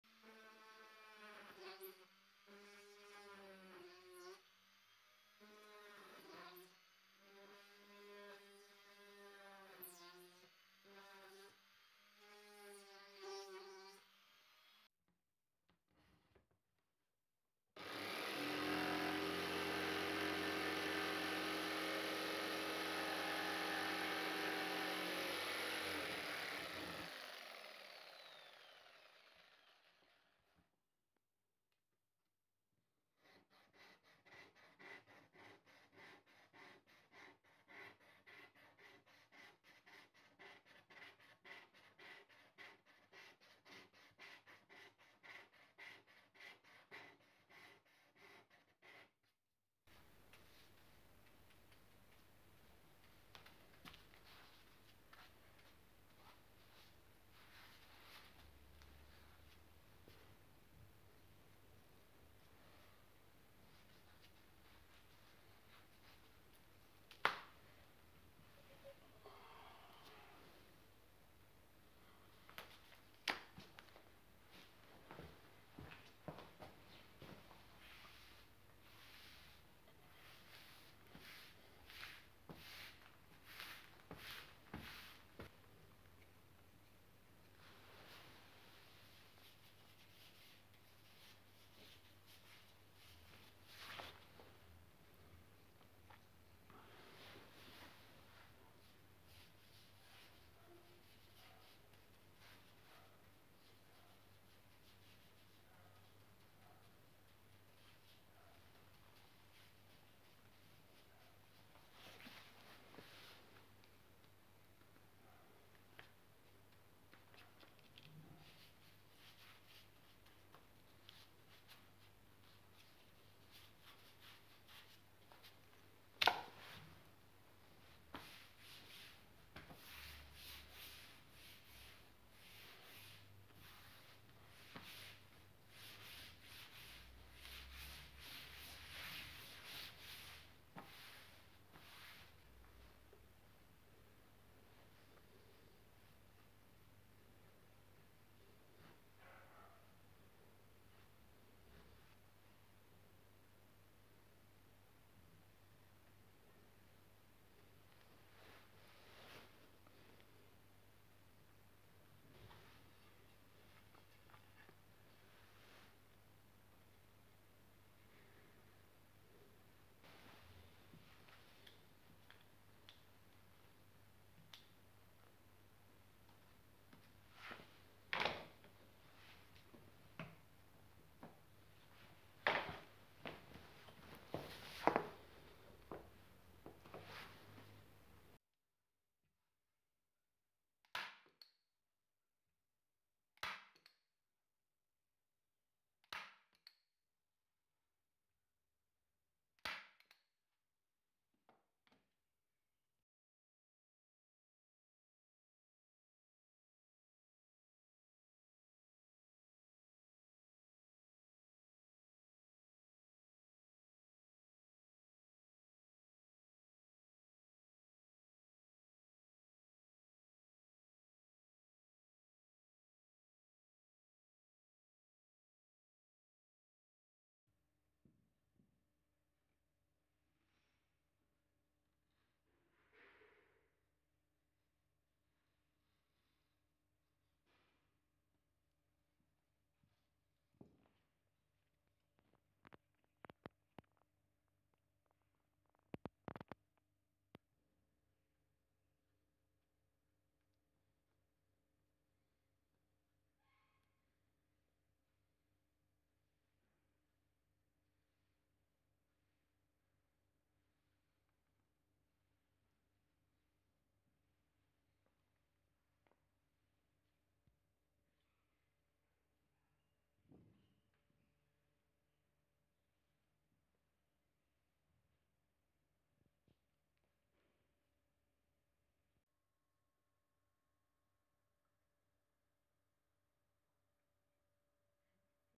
Costruire un medagliere
Costruzione di un medagliere 100cm x 90cm per un totale di 32 postazioni distribuite in 4 file da 8.